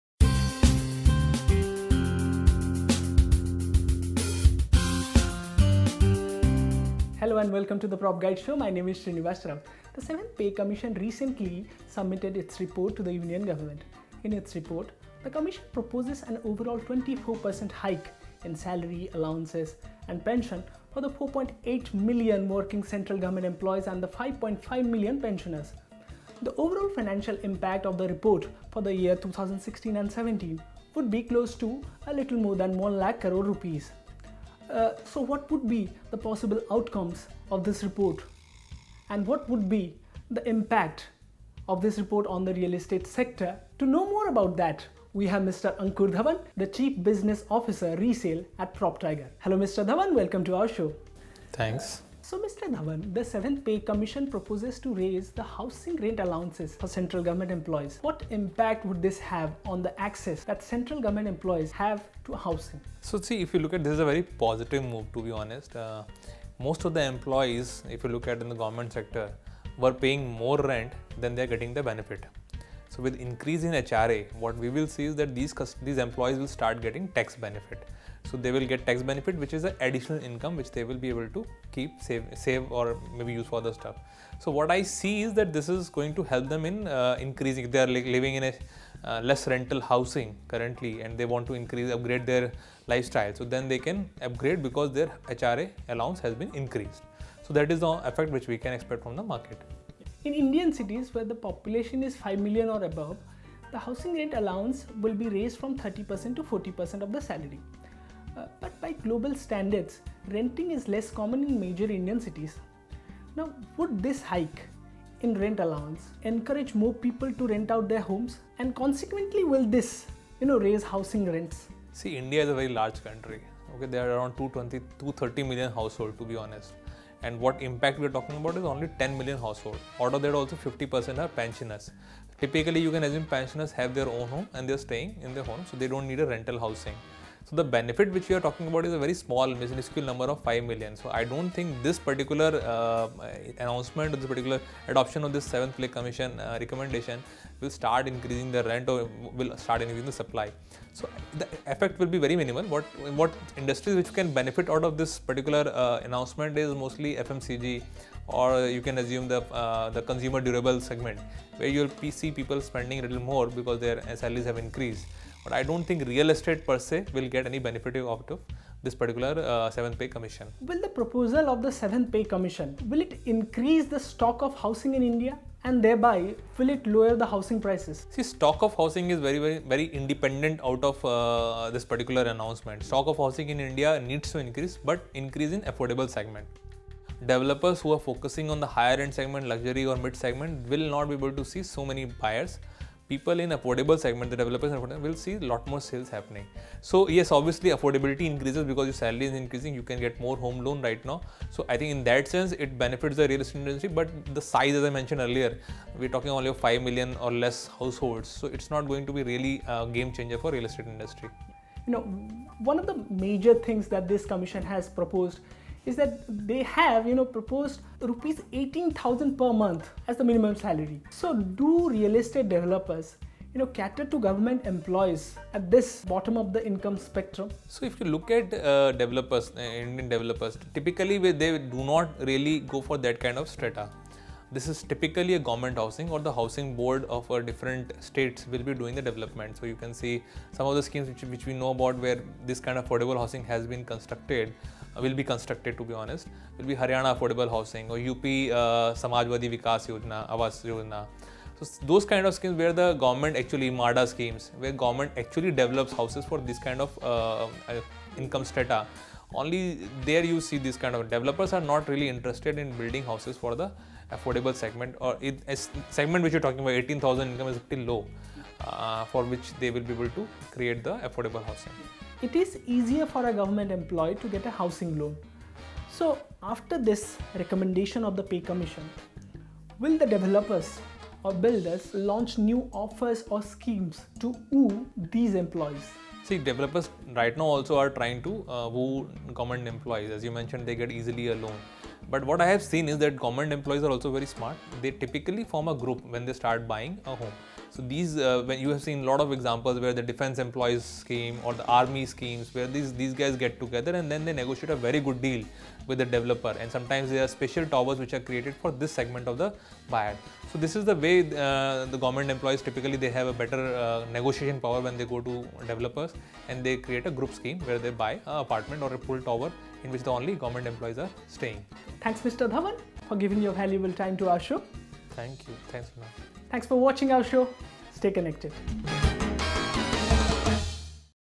Impact of 7th Pay Commission on Indian Real Estate Industry
The seventh pay commission proposes to raise the housing rent allowance (HRA) for central government employees. Ankur Dhawan, Chief Business Officer, PropTiger.com discusses the impact this would have on the access central govt employees have to real estate.